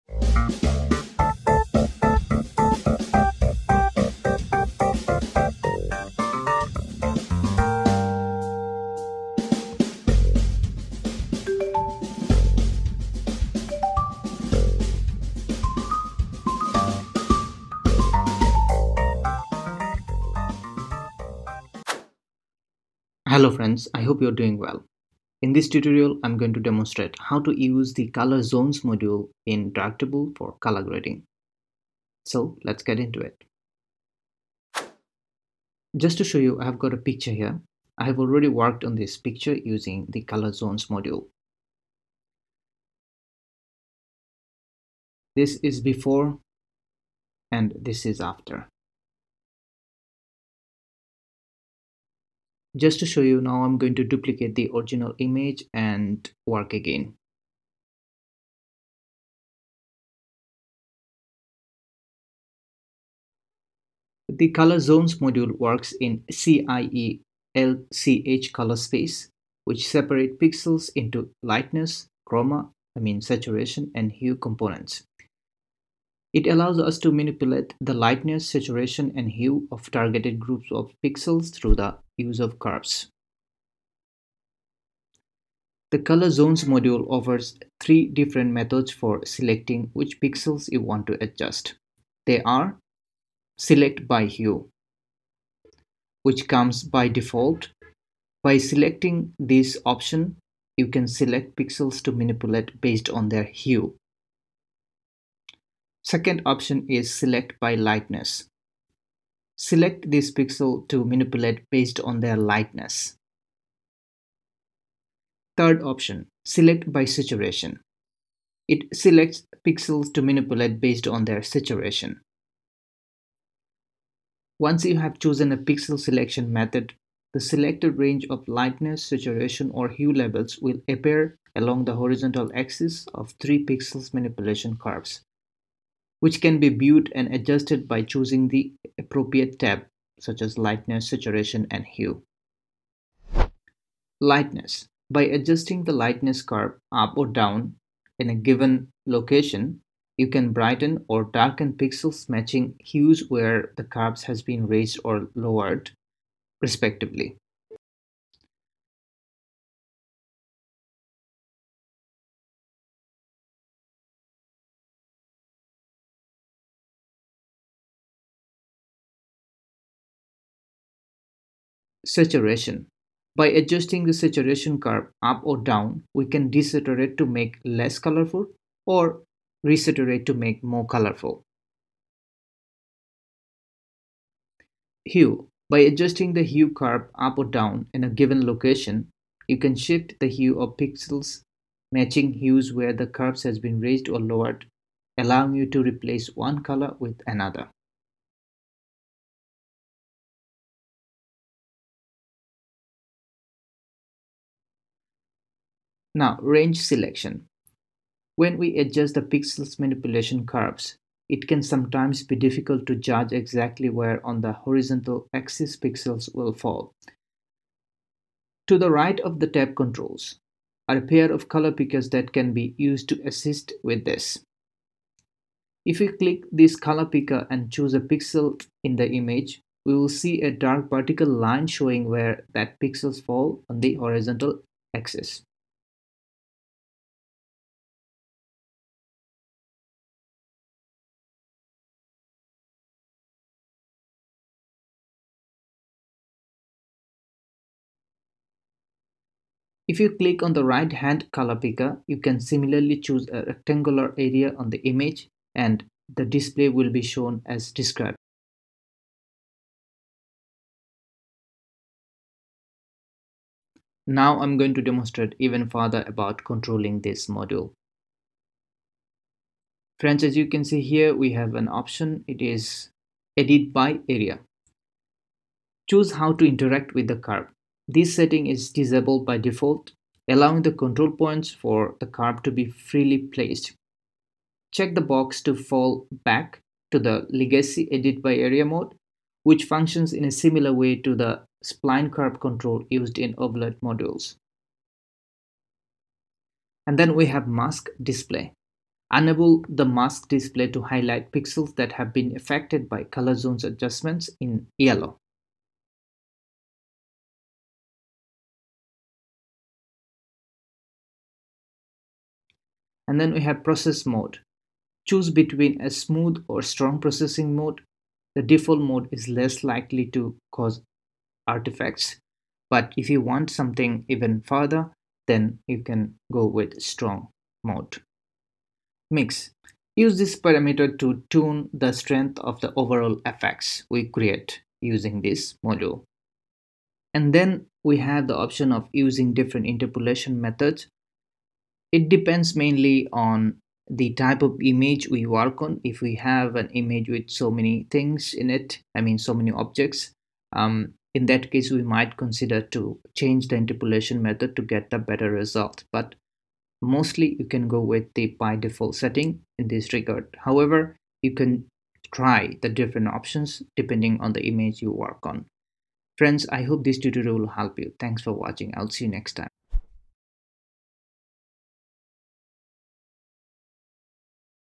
How to Use Colour Zones Module for Colour Grading | Darktable Tutorial
Hello friends,

Welcome to yet another Darktable Tutorial.

In this lesson, I am going to demonstrate everything about colour zones module. Please consider to watch entire video to understand everything fully.

Thanks
LI